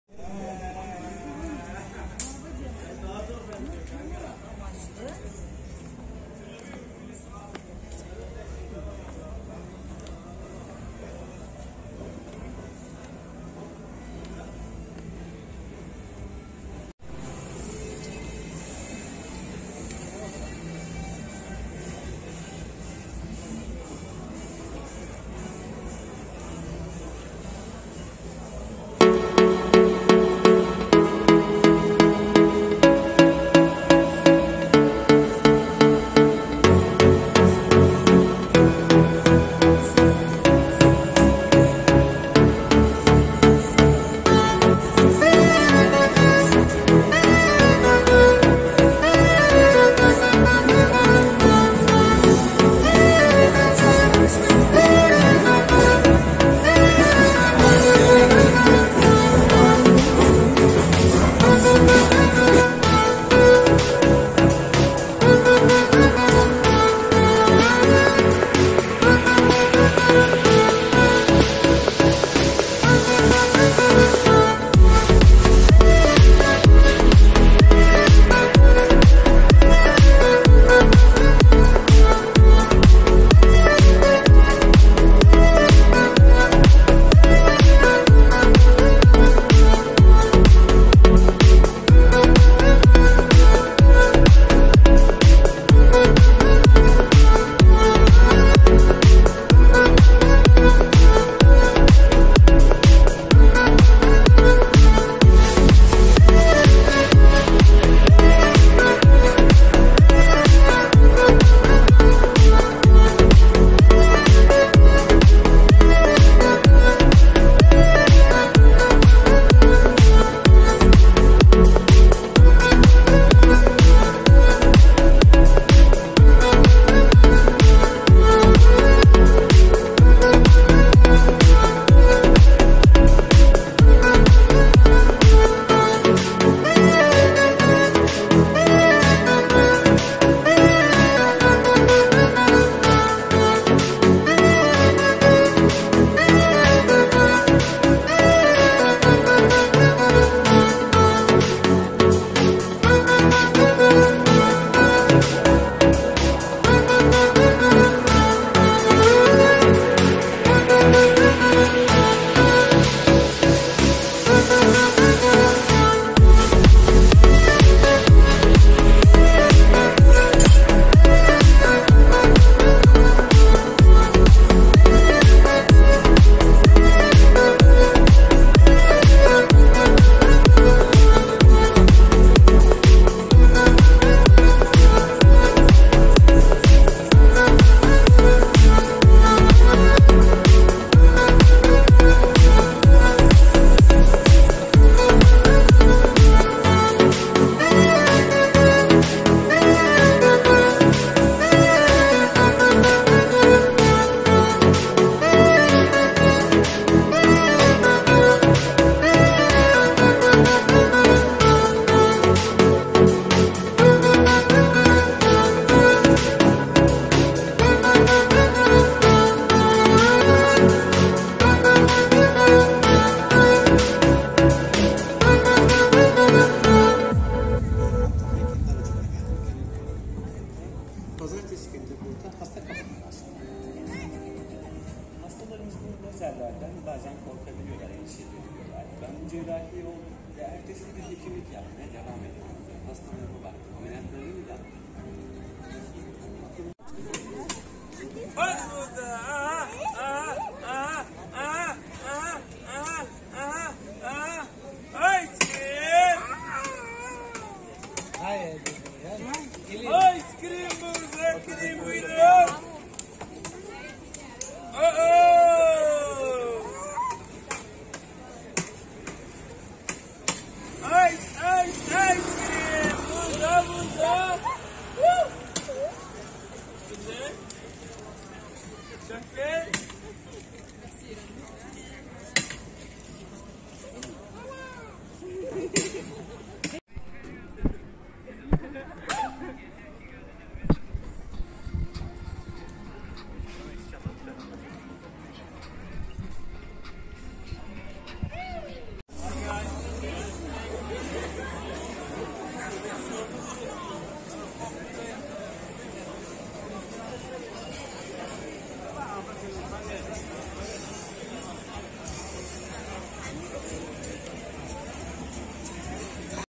ANTALYA KALEİÇİ AKŞAM VAKTİ
Merhabalar ben Yusuf Haktan bugün akşam vakti kaleiçine gittim. Bu güzel ve değişik görüntüleri sizlerle paylaşmak istedim. İyi seyirler.